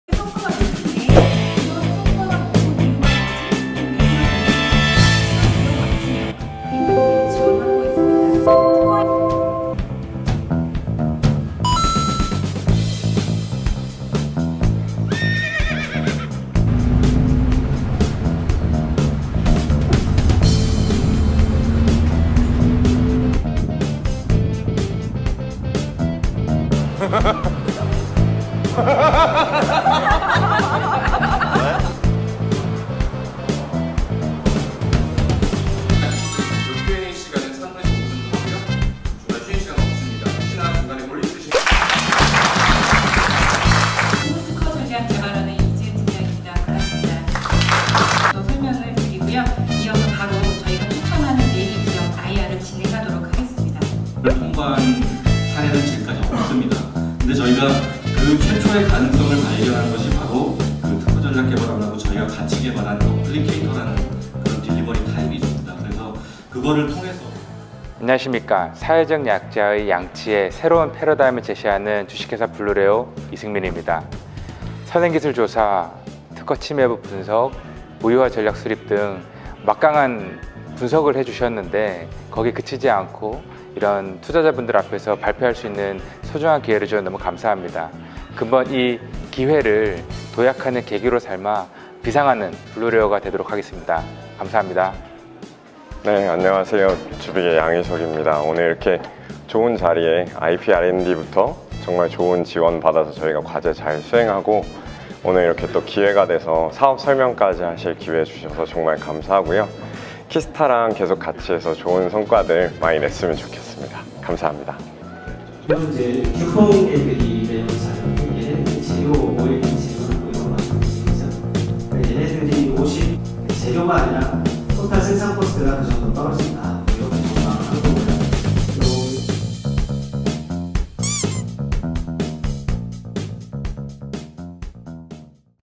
[찾아가는 키스타] NextRound 'IP-R&D Day'
국민 눈높이에 맞춰 키스타가 직접 행사 현장을 찾아 알려 드립니다.

2019년 9월 3일 KDB기업은행 본점에서 개최된
NextRound 'IP-R&D Day' 현장을 찾았습니다.

영상을 보시고 괜찮다😀~ 재미있다😁~ 볼만하다🤩~ 싶으면
좋아요👍와 채널 구독😍 부탁을 드립니다🙏

해당 행사에 관하여 궁금하거나, 자세한 안내를 원하시면 아래 댓글 또는 해당 사업팀 한국특허전략개발원 글로벌성장팀(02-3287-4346)으로 연락주세요 감사합니다.